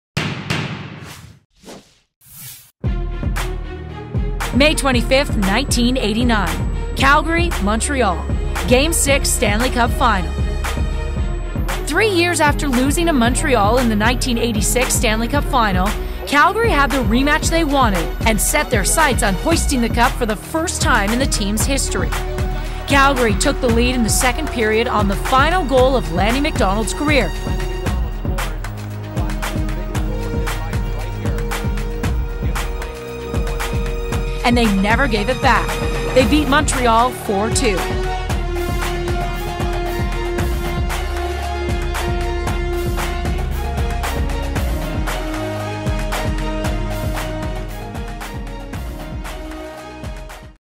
What a way to go out
Lanny McDonald scores and raises the Stanley Cup